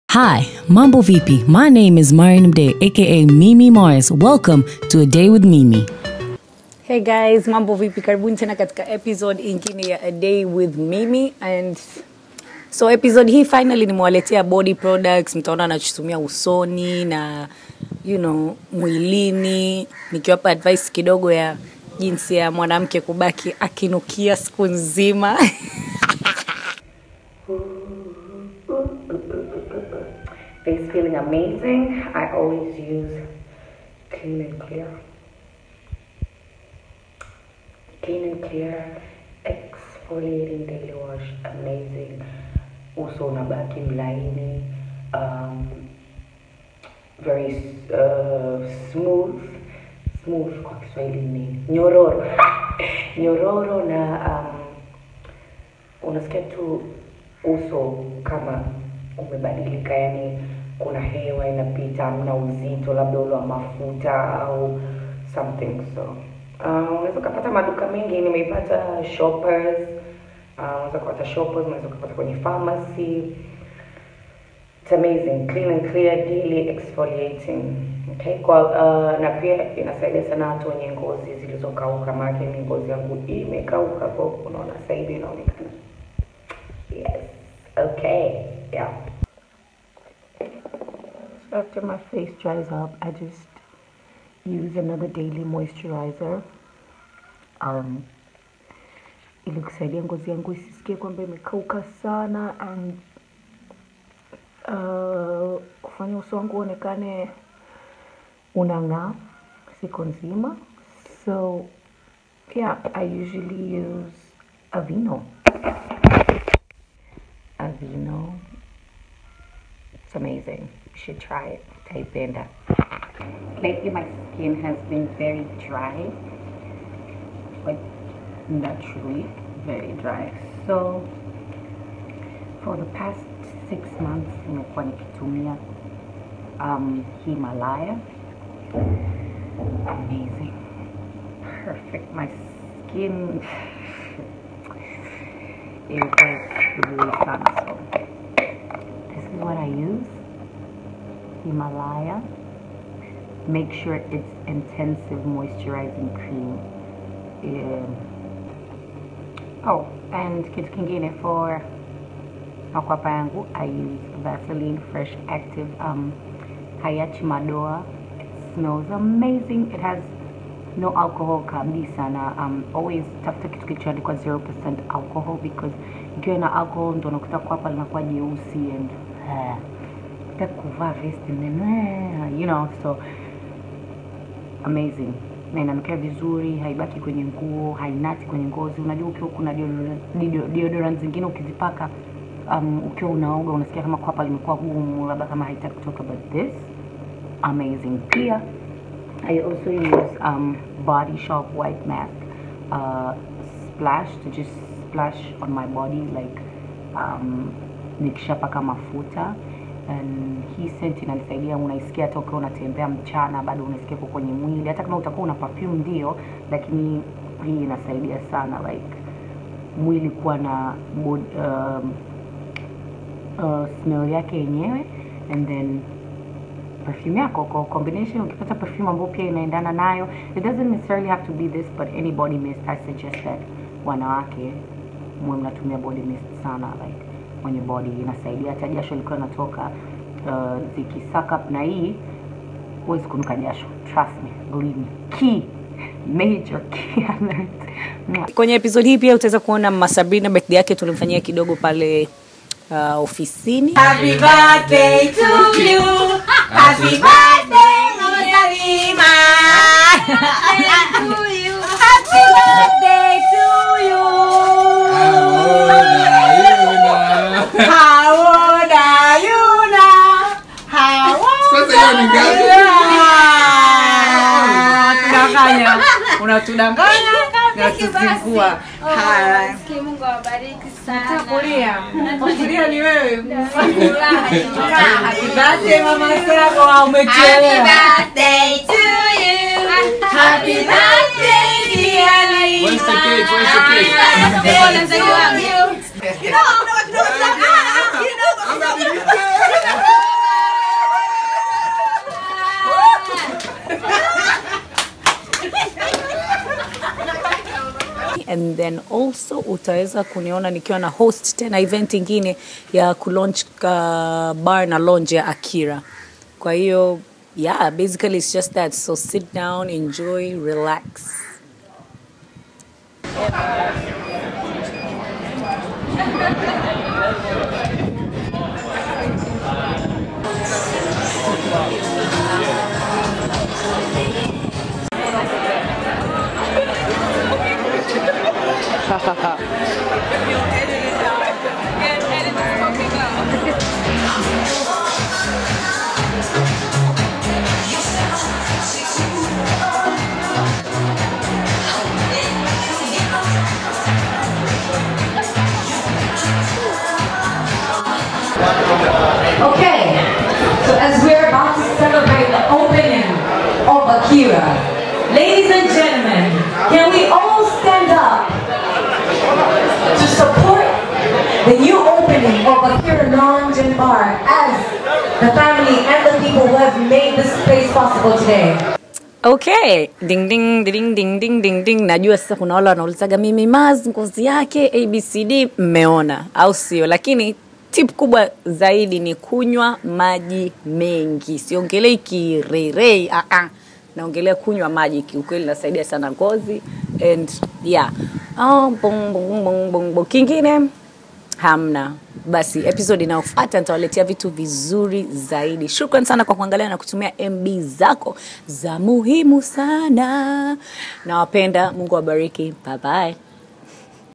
A day with Mimi Mars SE01EP08
On this episode Mimi shows you a few of her body products that she uses to keep her skin going and looking fresh and glowing.
FACE:
 CLEAN & CLEAR EXFOLIATING DAILY WASH
 [then after letting your face air dry or patting it dry whichever you choose]
AVEENO ACTIVE NATURALS DAILY MOISTURIZER

BODY:
HIMALAYA HERBALS INTENSIVE MOISTURIZING CREAM
VASELINE ACTIVE FRESH DEODORANT
THE BODY SHOP WHITE MUSK SMOKY ROSE FRAGRANCE MIST
Also on this episode you get to see a bit of Mimi hosting the AKIRA BAR & LOUNGE Launch.

Producer and Directed by Mimi Mars
Shot and Edited by Becka64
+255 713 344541